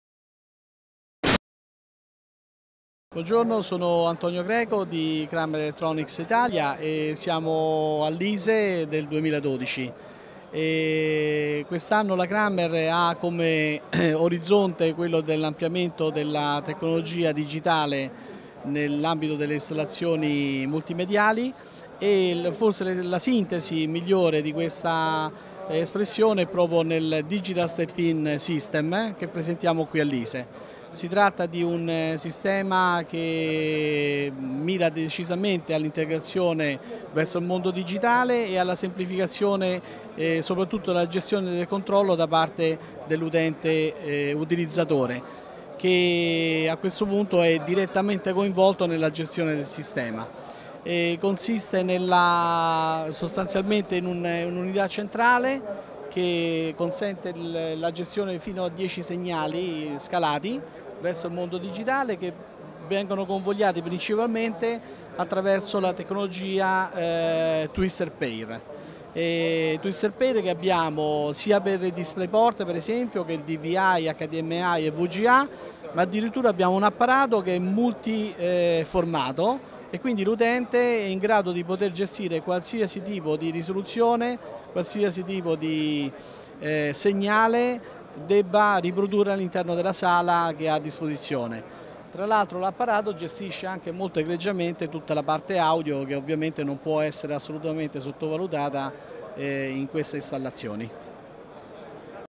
ISE 2012 - Intervista a Antonio Greco di Kramer Electronics Italia
Antonio Greco, Area Manager CSI di Kramer Electronics Italia, presenta il nuovo sistema Digital Step-In di Kramer.

Speciale ISE 2012 (31 Gennaio - 2 Febbraio 2012, Amsterdam RAI, NL) - Tutte le novità in diretta dall'Integrated Systems Europe, la fiera più importante a livello europeo per i settori dell'AV professionale e dell'integrazione di sistemi.

ISE 2012 - Interview to Antonio Greco of Kramer Electronics Italia (Italian lang.)

Antonio Greco, Area Manager CSI at Kramer Electronics Italia, present the new Digital Step-In introduced by Kramer at ISE.